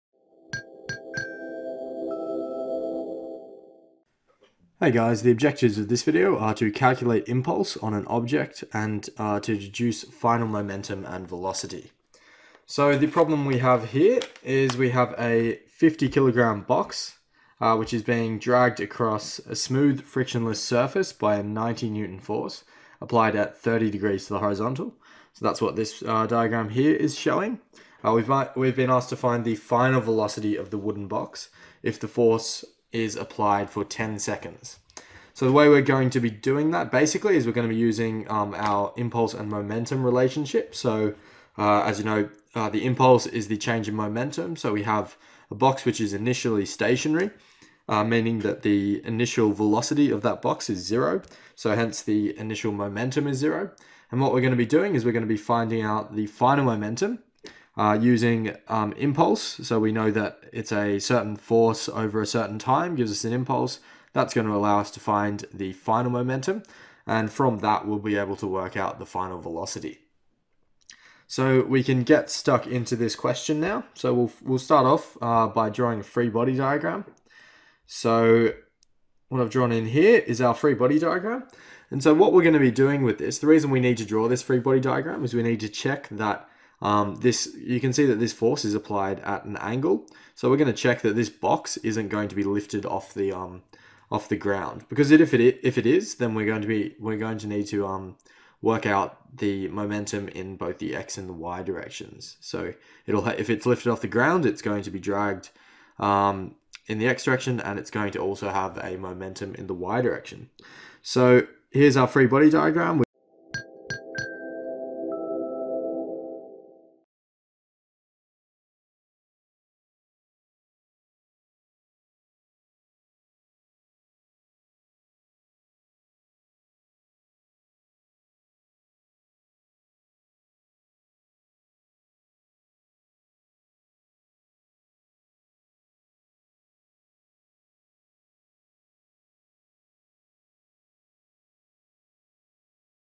Impulse, Momentum, Velocity Example 1 | Dynamics
for more FREE video tutorials covering Engineering Mechanics (Statics & Dynamics)

The objective of this video is to calculate impulse of an object to deduce final momentum & velocity. First part of the video jumps straight to the given exemplary problem where it has shown that a box of 50 kg mass is dragged across a smooth, frictionless surface by a 90 N force applied at 30 degree to the horizontal. The video asks to find out the final velocity of a wooden box if the force is applied for 10 seconds. The gravitational acceleration is given as 10 m/s2.

Moving on, the video explains the given schematic diagram of the problem and shows how to draw the free body diagram elaborating all the facts & figures in greater details. Next, the video resolves force components in y direction to find the value of normal force. Later, the video uses the obtained value as input to the integral formula of impulse & thus calculates the value of impulse. Finally, the video uses the impulse and momentum equation to find out the final velocity of the wooded box.